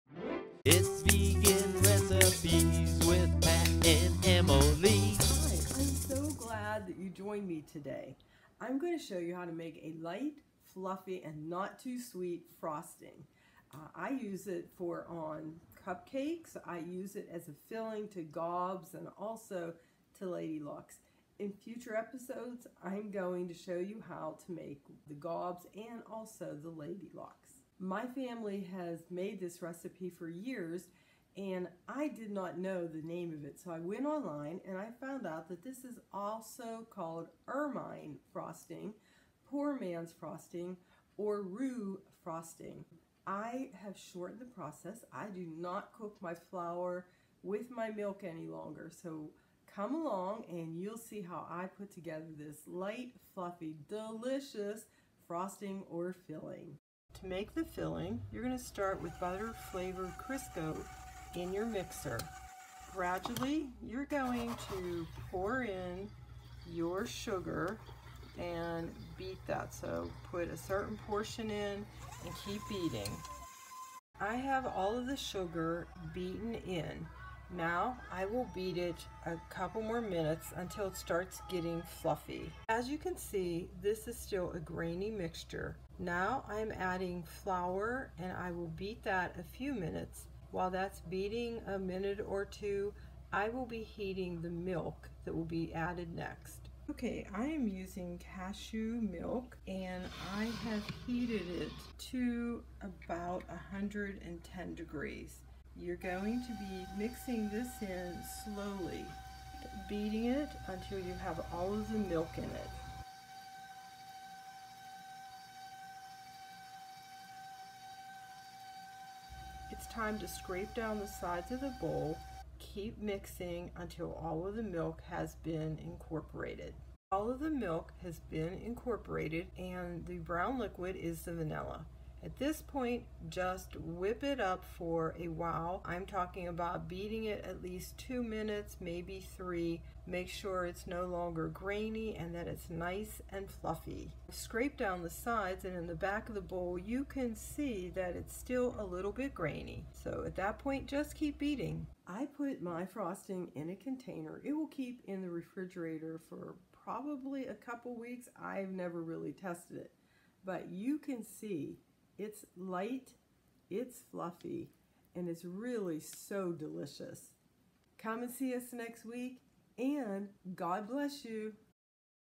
Ermine, Roux, Flour, and/or Poor Man’s Frosting, Gob Filling and Lady Locke filling
Recipe makes 2 cups.
This frosting and filling has many names!
Ermine
Poor Man's
Roux
Gob Filling
Lady Locke filling

Best to use a stand mixer with a wire whip attachment but you can still make this with a handheld mixer but it will take longer.

1 cup butter-flavored Crisco
1 cup white granulated sugar
1/2 cup plant-based milk
2 tablespoons flour
1 tablespoon vanilla

Whip the Crisco and sugar together.  Beat until getting creamy. Add flour and mix well. Heat plant-based milk to 110 F. Slowly add the milk in small portions. Whipping after each addition until all the milk has been added. Add the vanilla. Whip until light and fluffy and until all the sugar is dissolved.

This frosting stores well in the refrigerator and freezer.